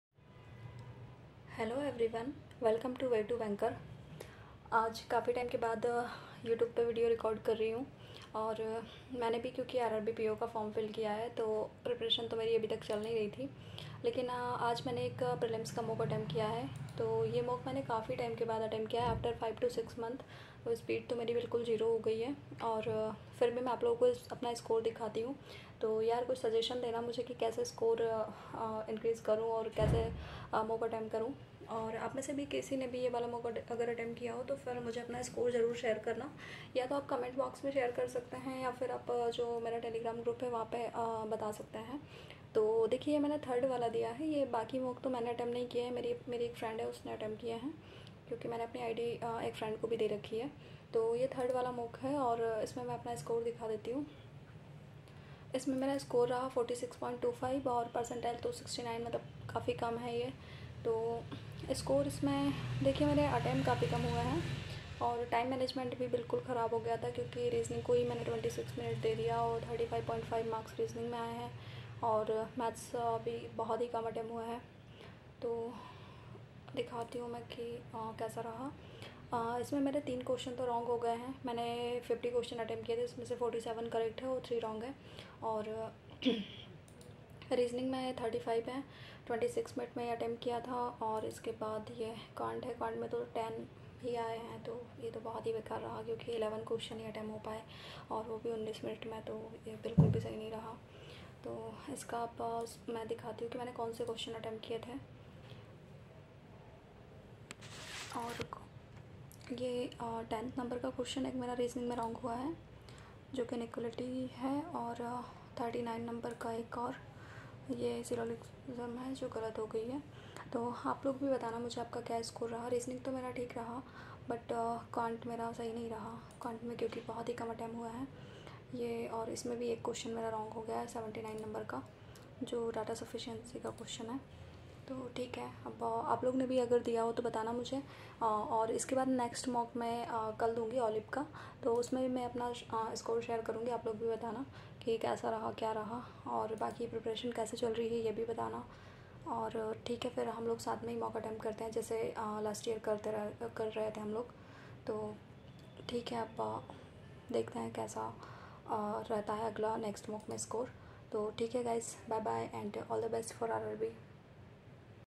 My marks in today's mock (RRB PO 3)....Continue rahna boht jruri hai...
after a long time today I have attempt a mock of adda247... you can see my marks in this video